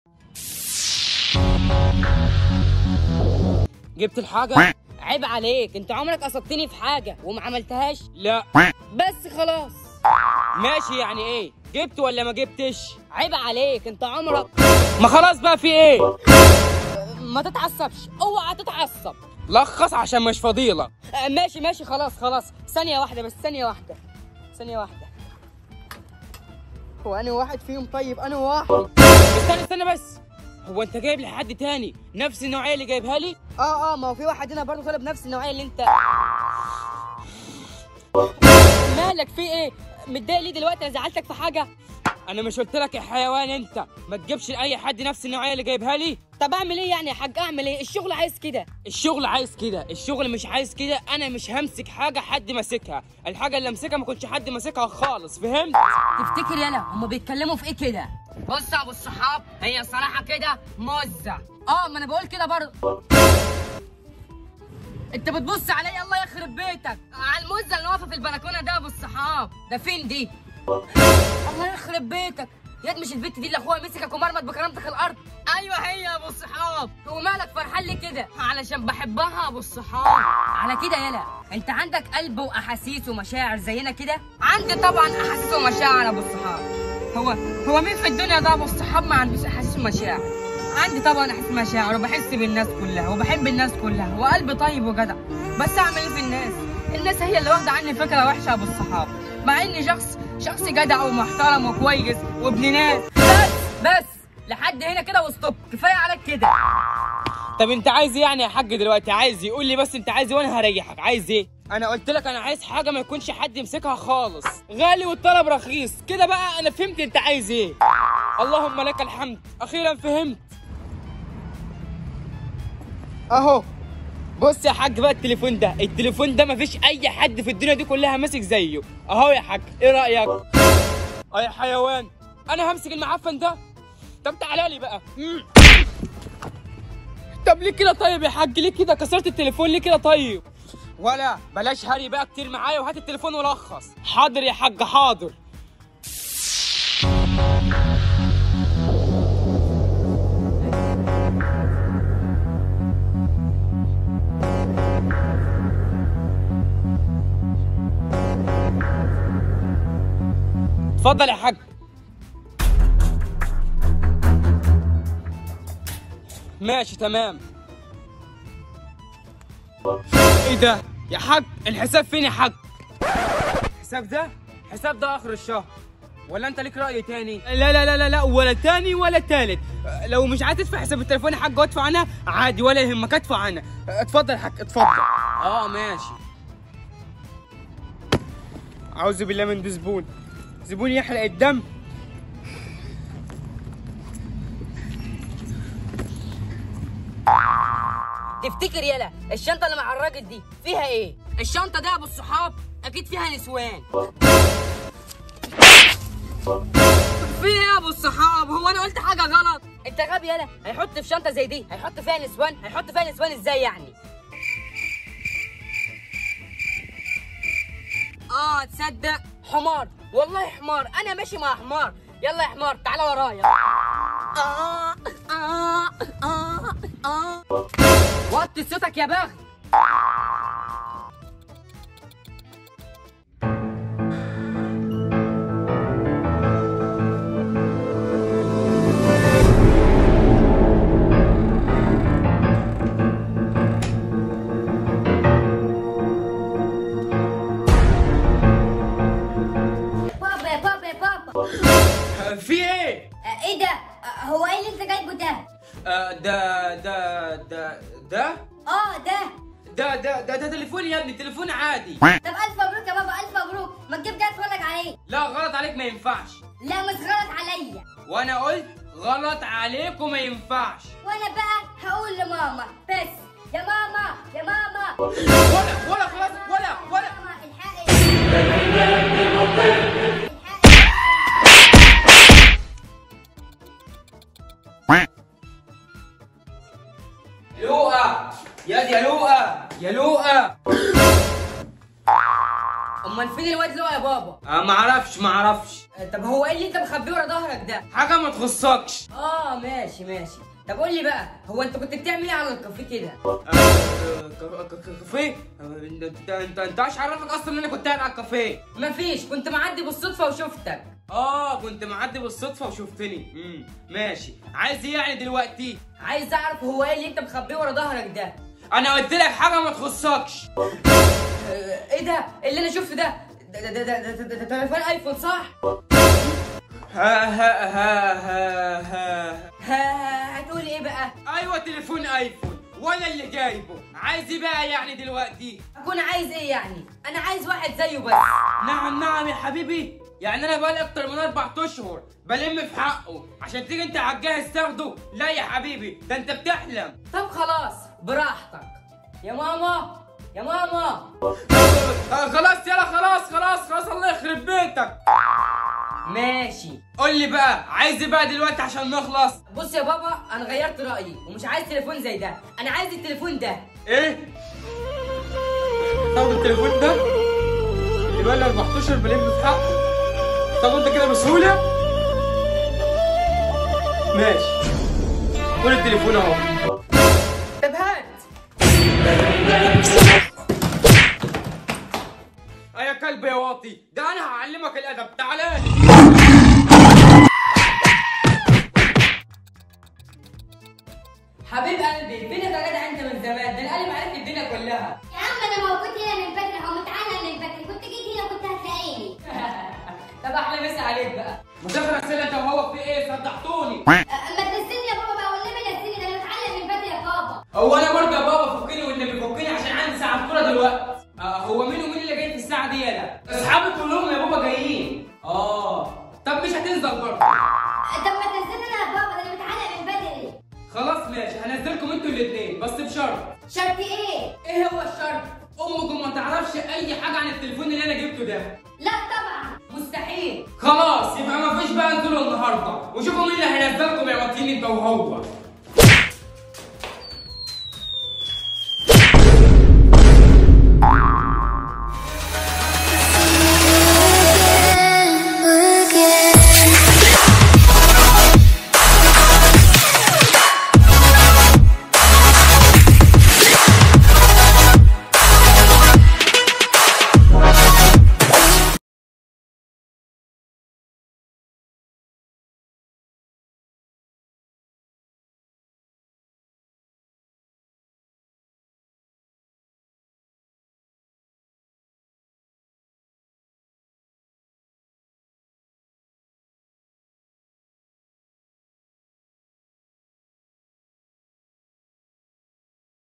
لما ابوك يجيب لنفسه تلفون ايفون 11 📲 وميعرفكمش 🤷‍♂️😂| علاء حسين
في القناة وتفعلوا زر الجرس 😍😍 وشكرا جدا للمشاهده😍